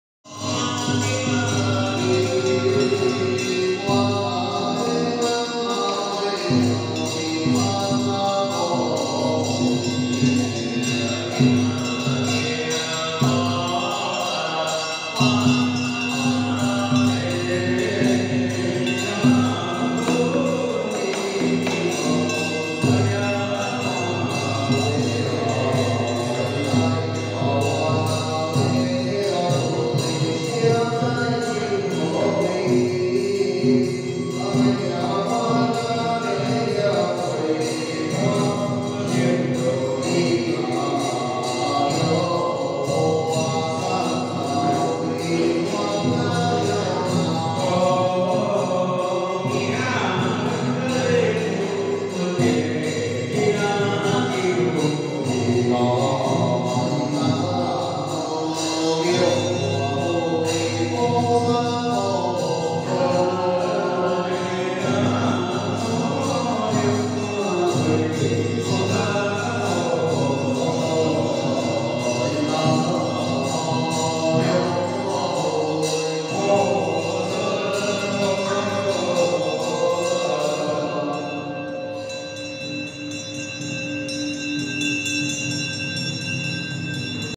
玉井明泉寺梁皇法會
立定法師領眾
志祥法師打鼓